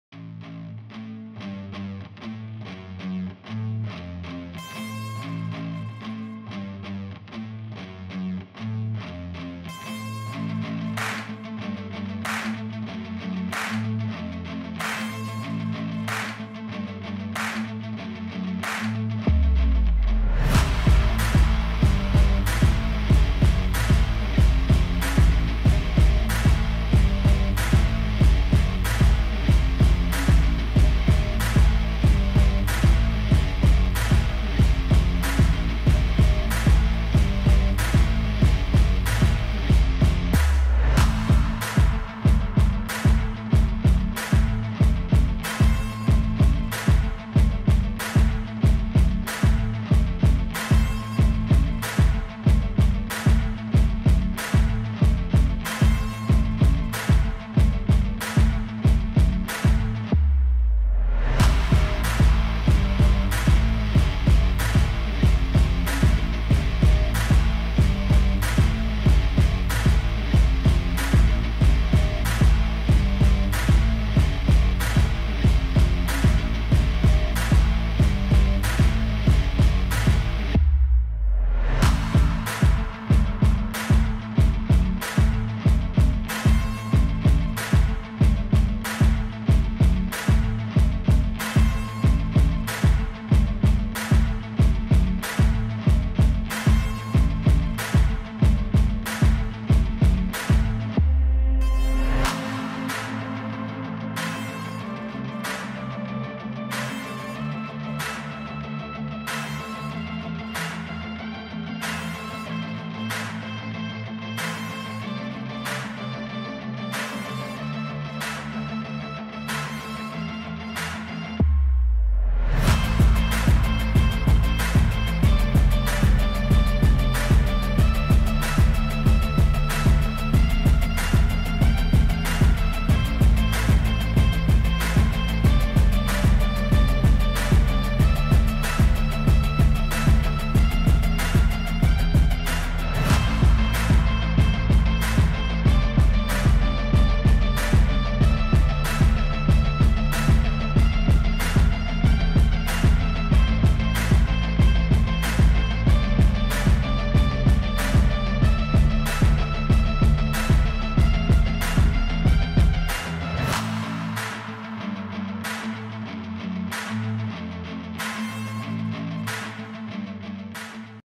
Scale ACT 2024 - Armour and Diorama Highlights
The ol' Lance Corporal got his arse up to the ACT to look in on Scale ACT 2024, considering we sponsored the 1/35 scale armour category it was a real joy to see the quality of work and breadth of models. Well done to every entrant.